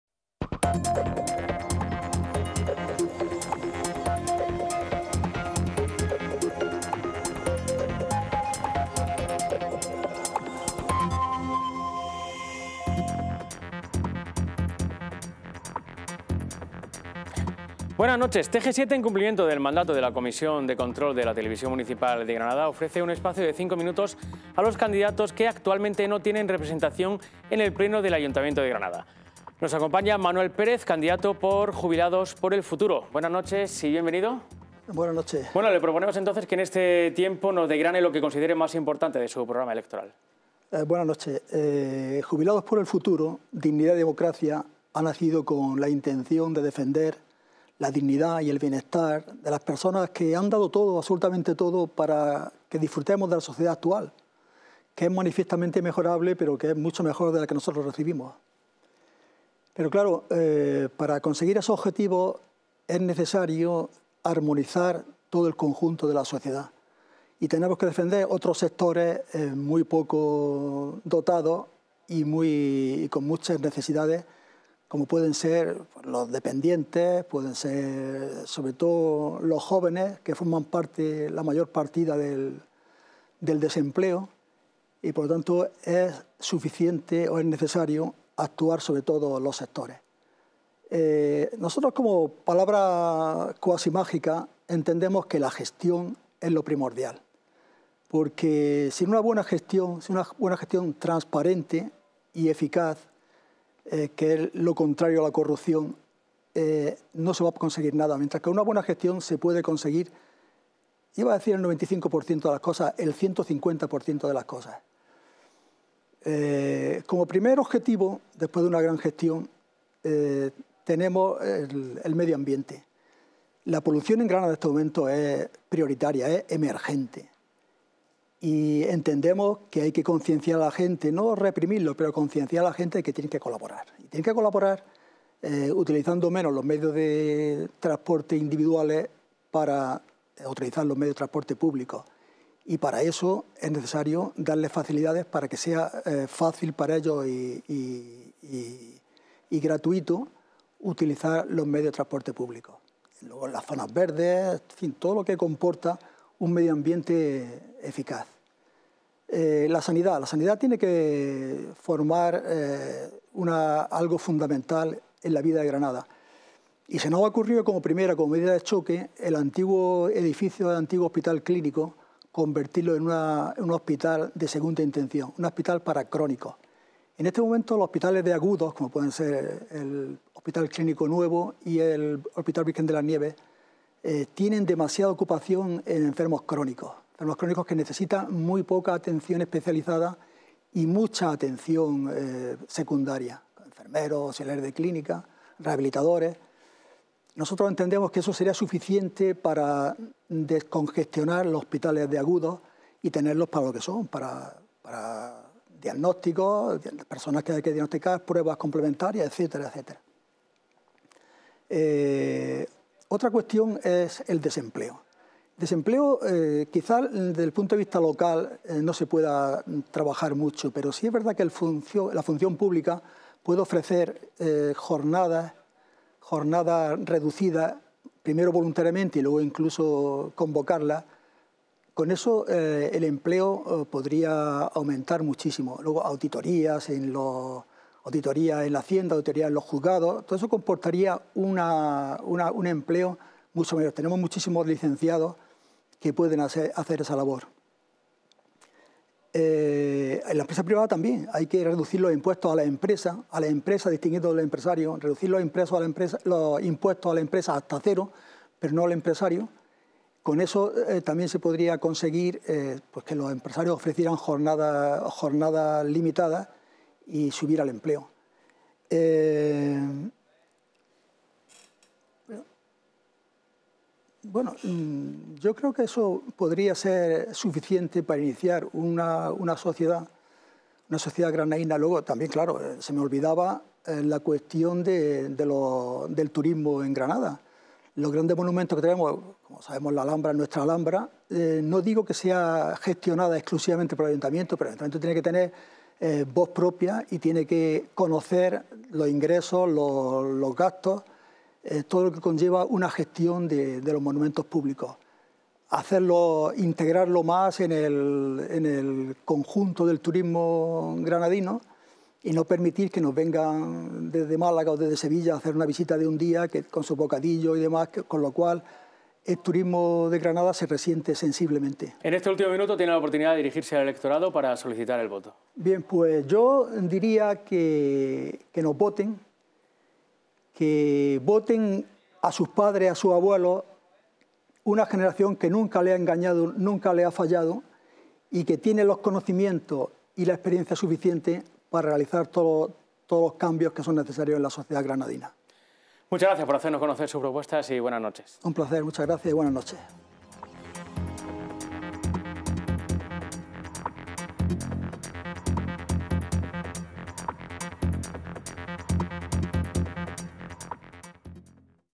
ENTREVISTA a MANUEL PEREZ, candidato de Jubilados por el futuro a la alcaldía de Granada
Hoy, Miércoles 15, a las 21:00 horas, Fran Latorre entrevista a Manuel Pérez, candidato de Jubilados por el futuro a la alcaldía de Granada